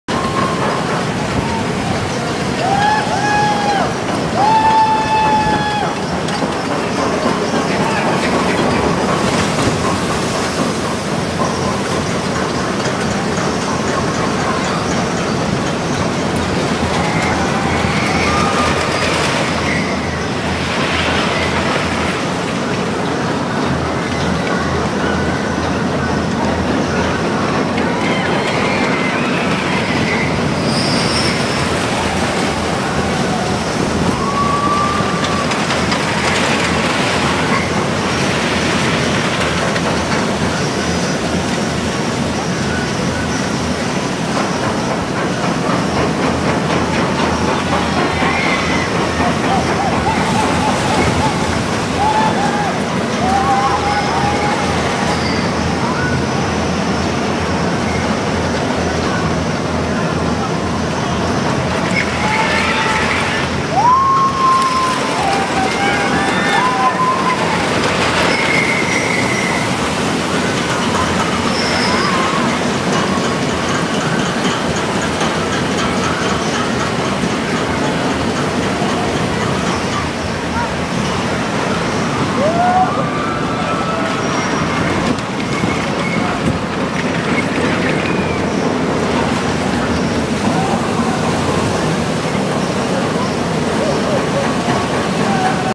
Cyclone Rollercoaster
The famed Cyclone ride at New York's Coney Island.